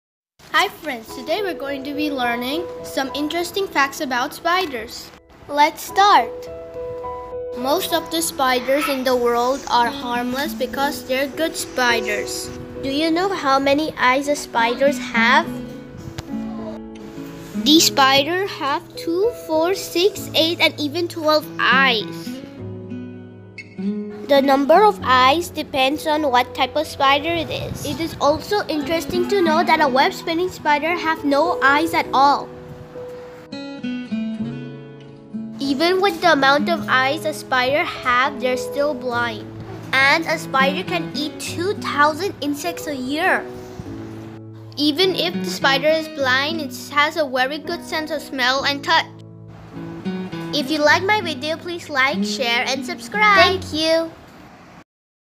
Spider- 6 Amazing facts||My first animation video
Spider- My first animation video about some interesting facts about spider